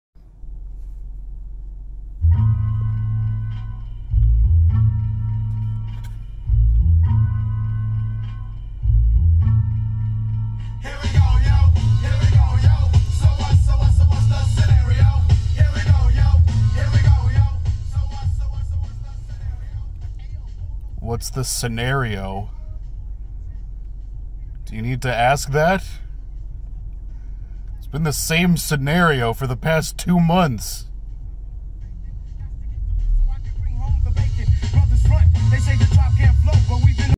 Sad Guy With a Beard wants to get down to Tribe Called Quest, but he can't because he's too sad.
I'm trying to enjoy my favorite funky beats, but the coronavirus has just really got me down.